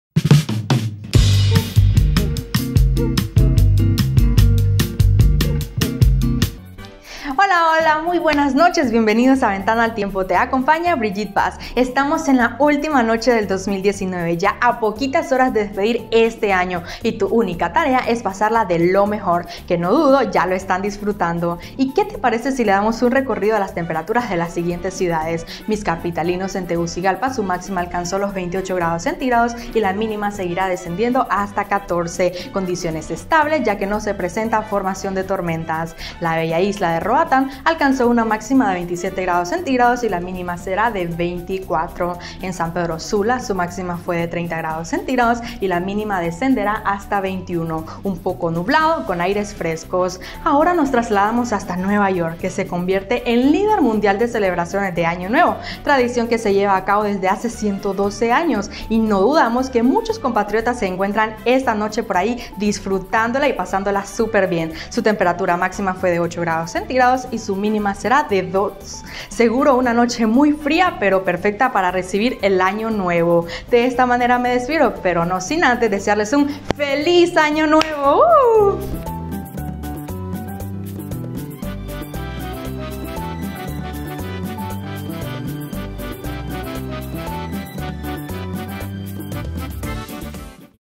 VENTANA AL TIEMPO: condiciones estables para casi todo Honduras
Se prevé que para el clima de esta noche, las temperaturas sean secas y estables en la mayor parte del territorio nacional.